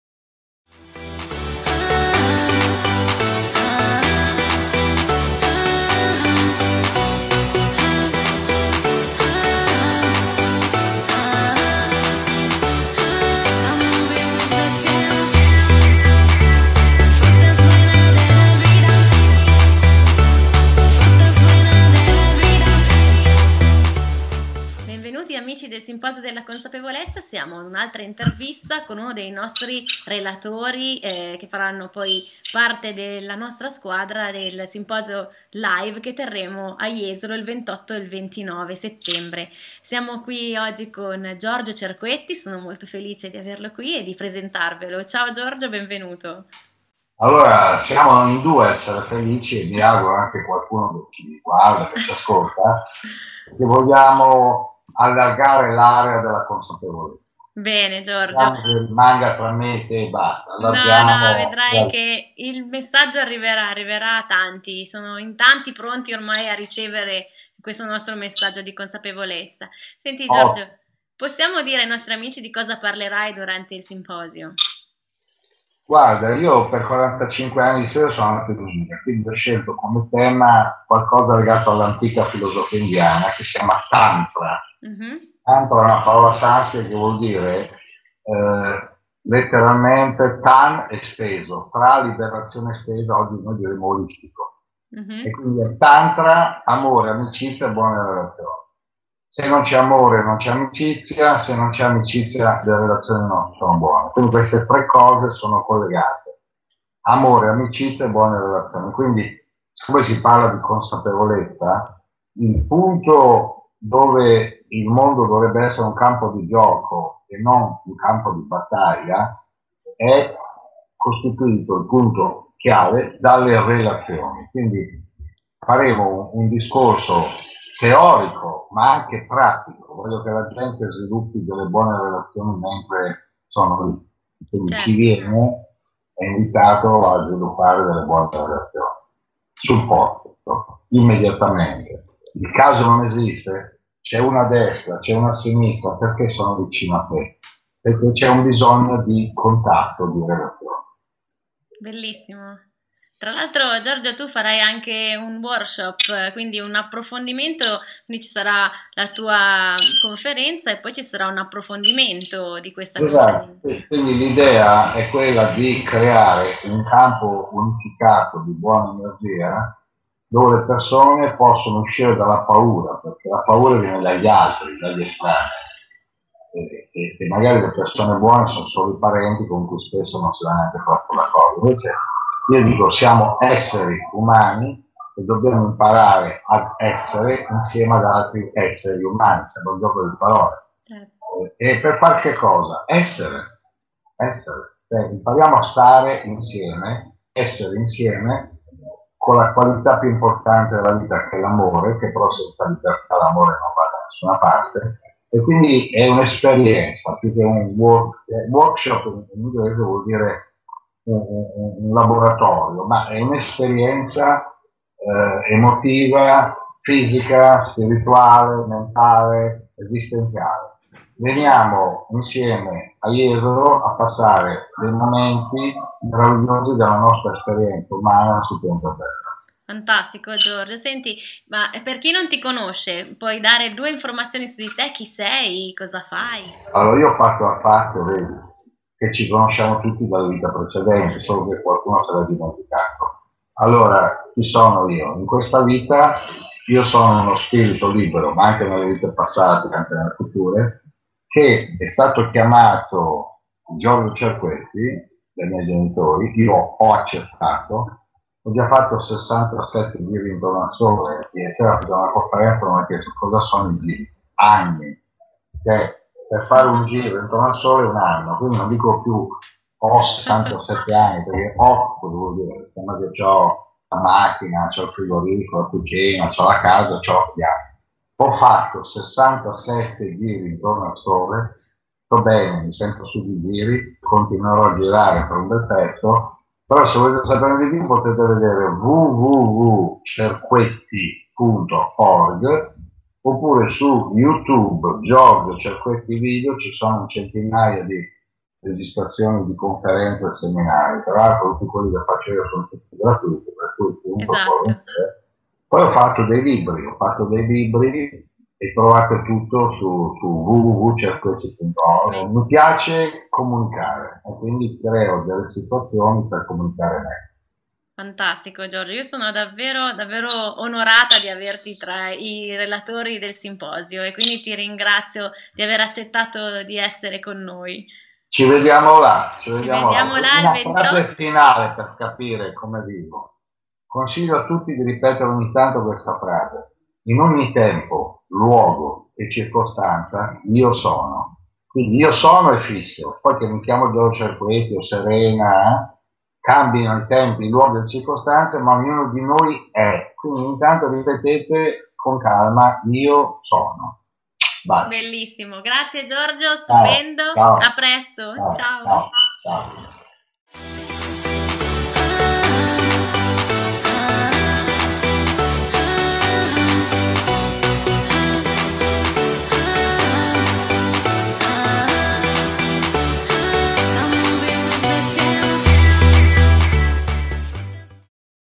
Giorgio Cerquetti - Intervista Flash
Video n°227 - Giorgio Cerquetti
09/09/2013 - Marina di Montemarciano - Ancona
Simposio Live 2013 Jesolo

Serena Sirini intervista GIORGIO CERQUETTI

Facebook: GIORGIO CERQUETTI TI AMA